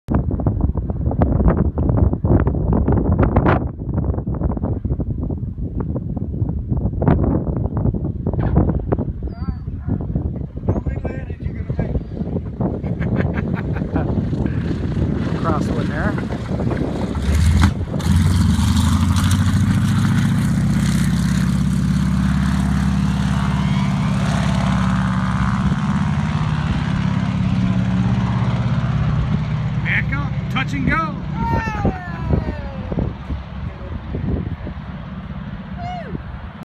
American Texan T6
Ian's little bit of a rough landing..